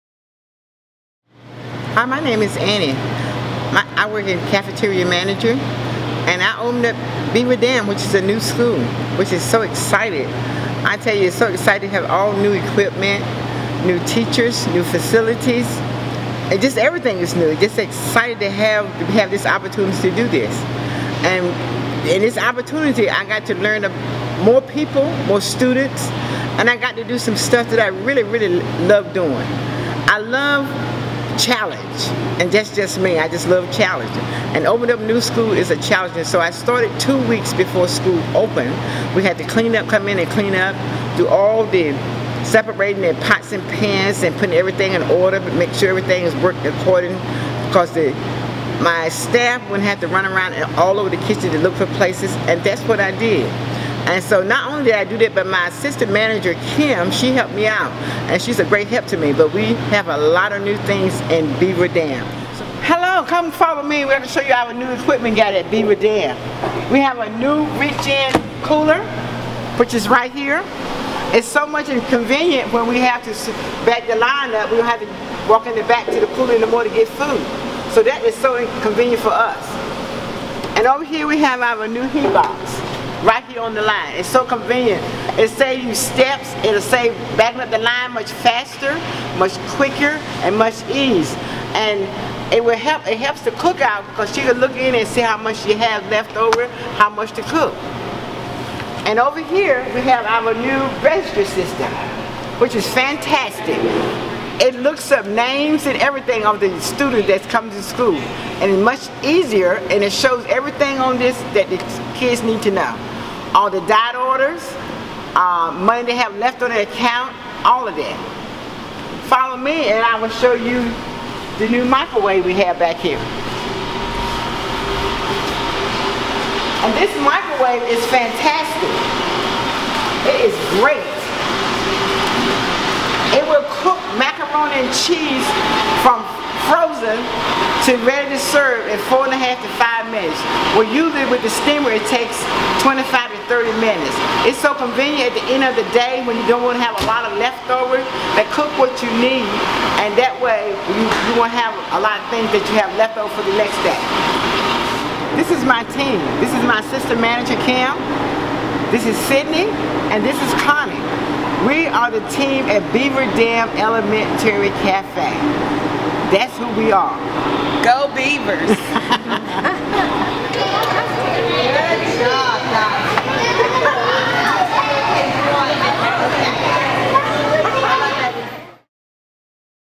Opening a new school's dining room
WCPSS Child Nutrition opens a  new dining room in each of its new schools. Annie Williams of WCPSS Child Nutrition opened the dining room at the new Beaverdam Elementary.